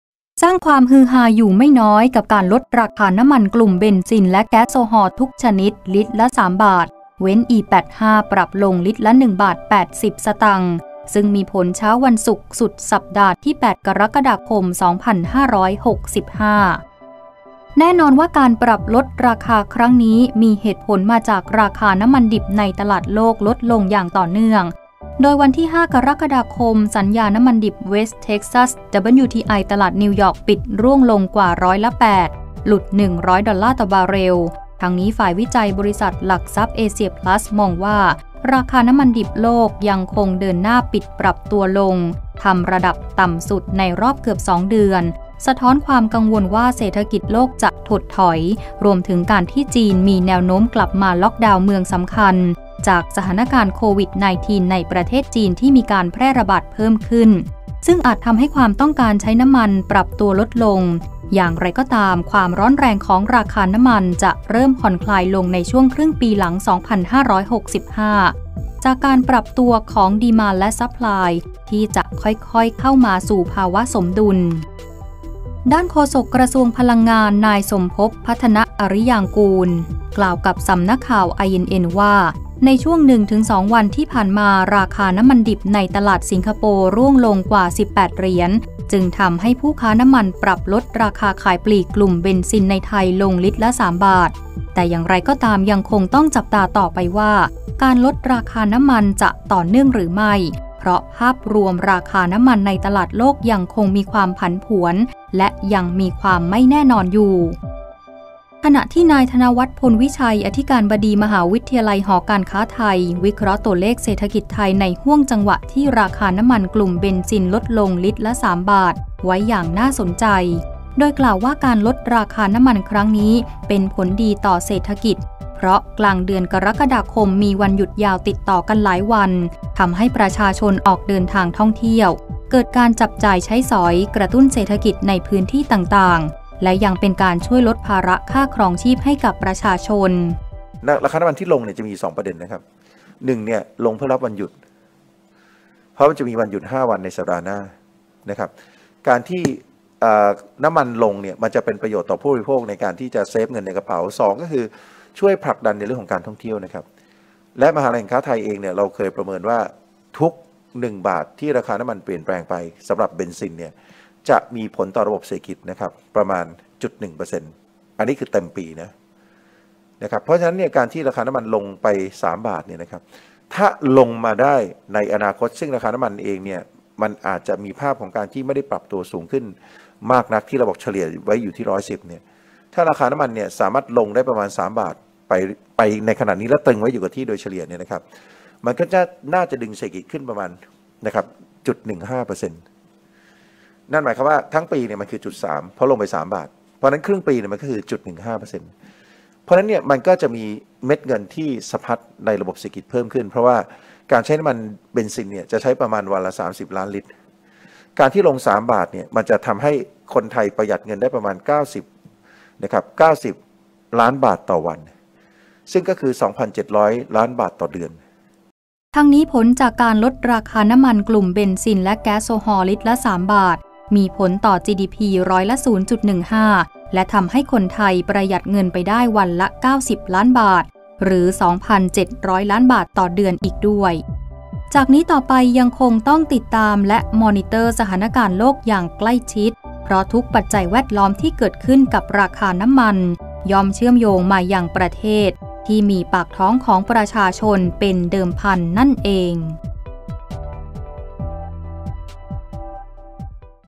กลุ่มเบนซินลด3บาท เงินสะพัดพาคนเที่ยว : 08-07-65 | iNN Top Story
สร้างความฮือฮาอยู่ไม่น้อยกับการลดราคาน้ำมันกลุ่มเบนซิน และแก๊สโซฮอล์ทุกชนิดลิตรละ 3 บาท เว้น E85 ปรับลงลิตรละ1.80 บาท ซึ่งมีผลเช้าวันศุกร์สุดสัปดาห์ที่ 8 ก.ค.2565